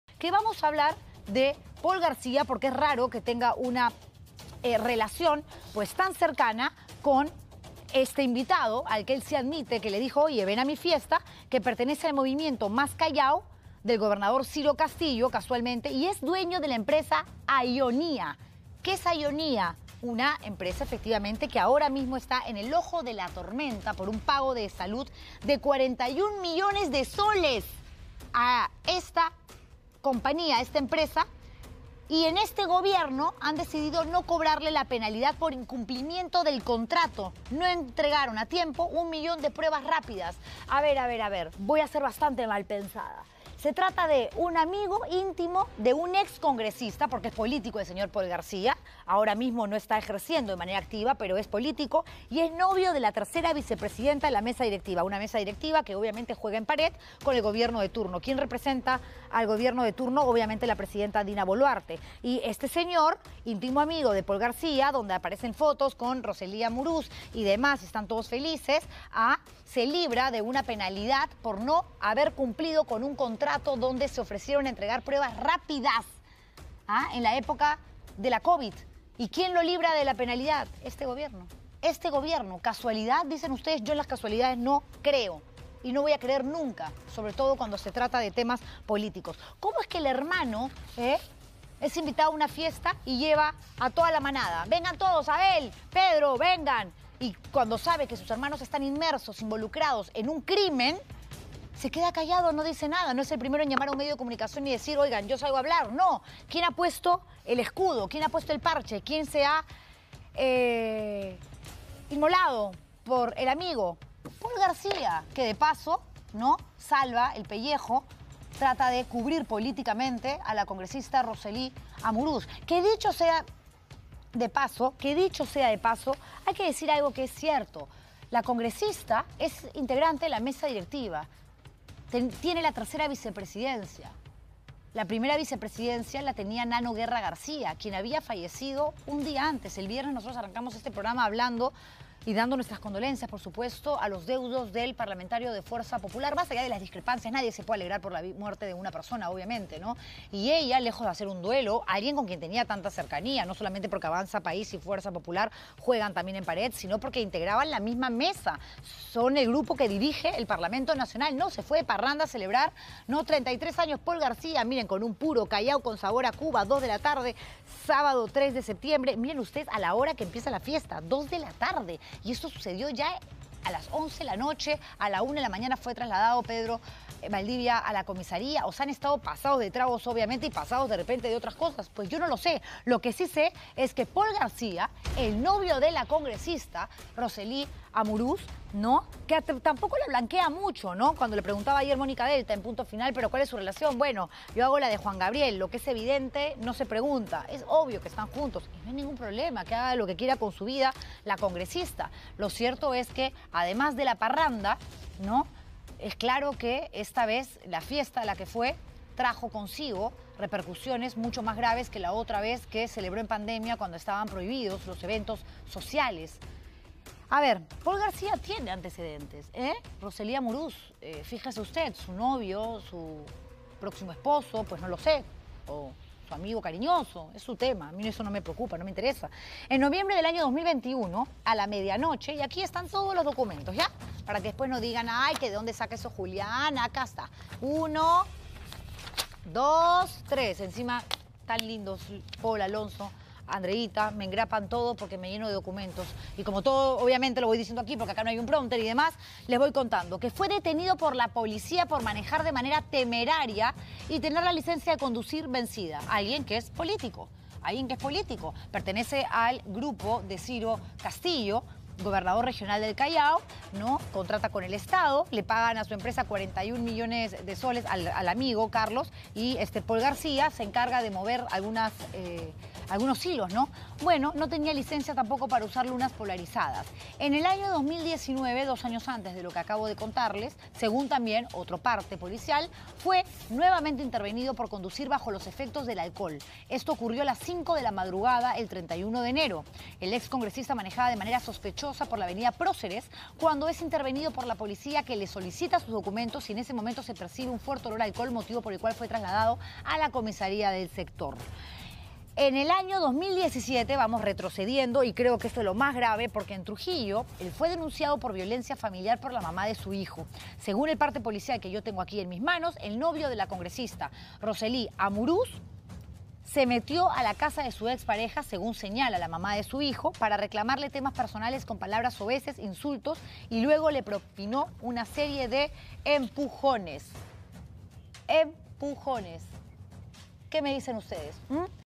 Paul García: Los antecedentes policiales del excongresista y pareja de Rosselli Amuruz
El exmiembro de la bancada de Acción Popular mantiene incluso una denuncia por violencia familiar.